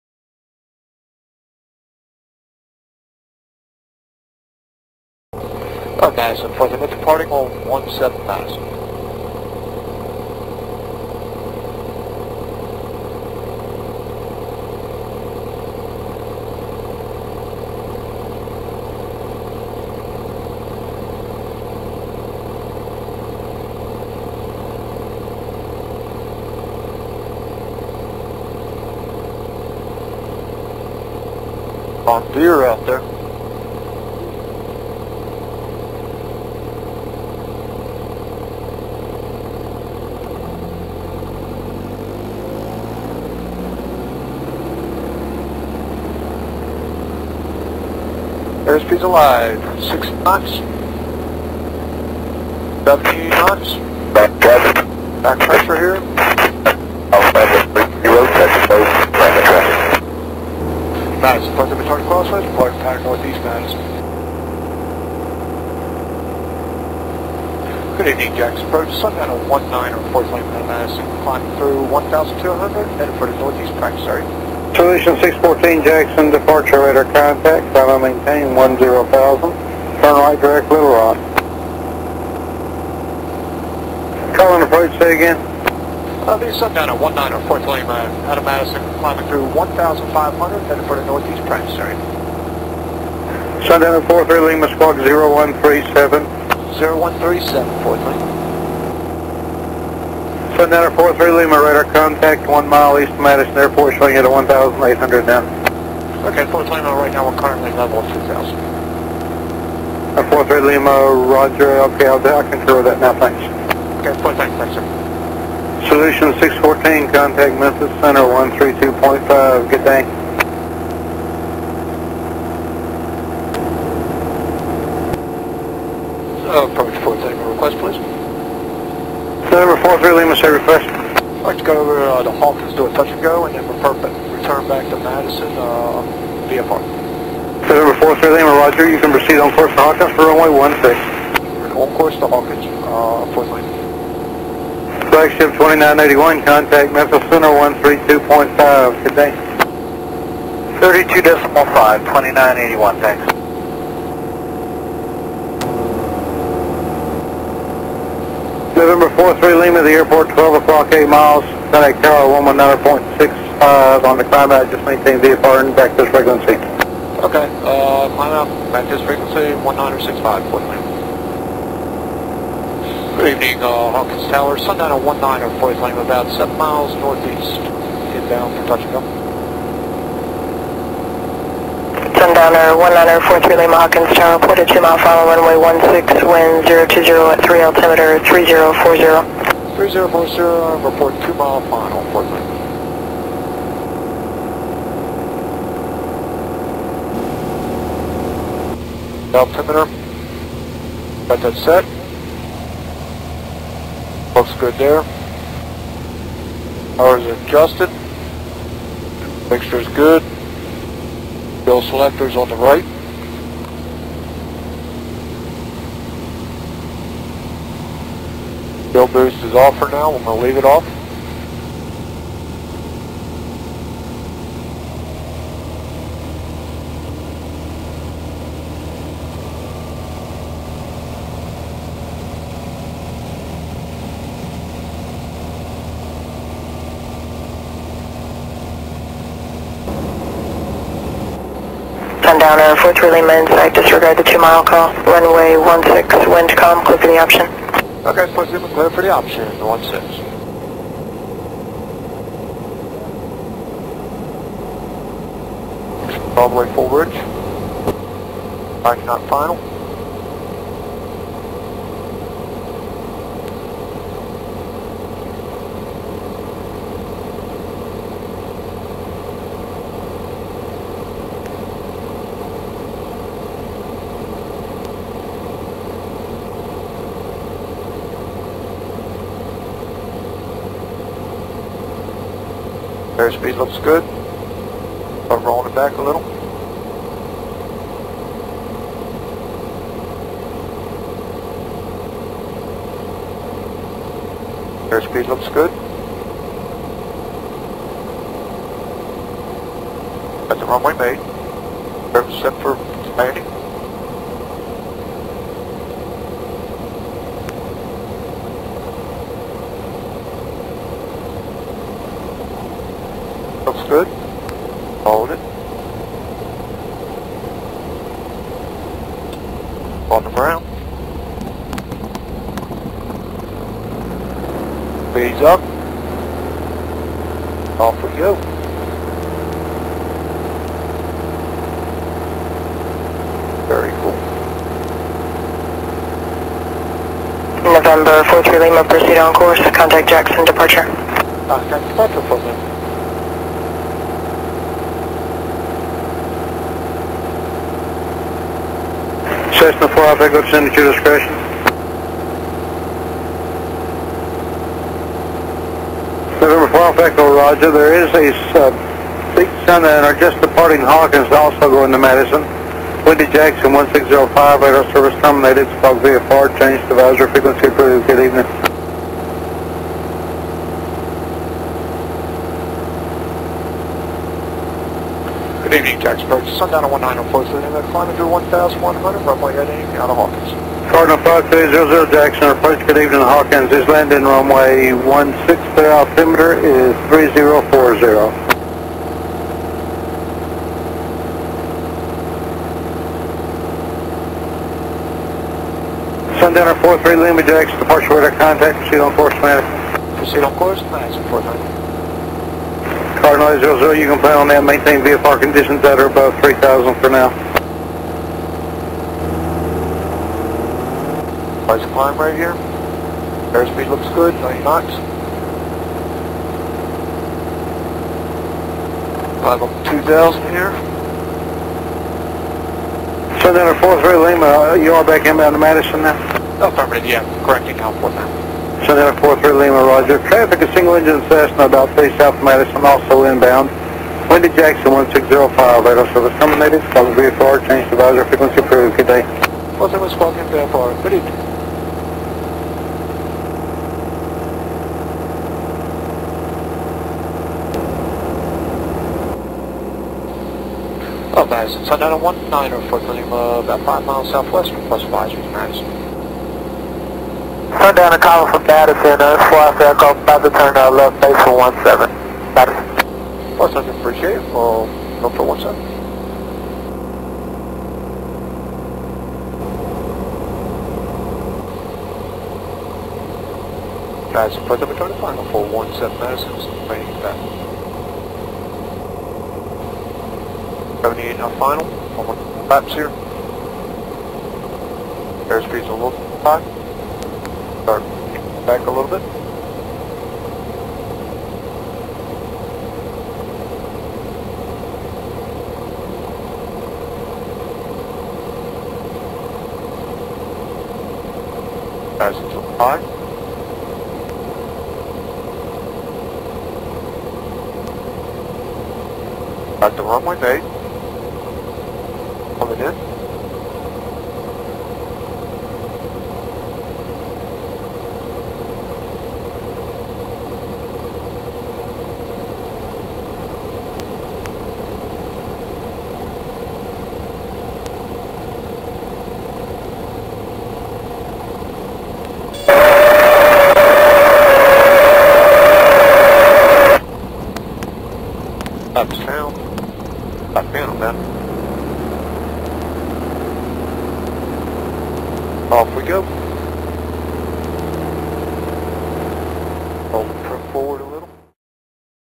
Night Landings at KHKS and KMBO with ATC COMMS
Night landings at Jackson Hawkins and Madison MS under VFR conditions.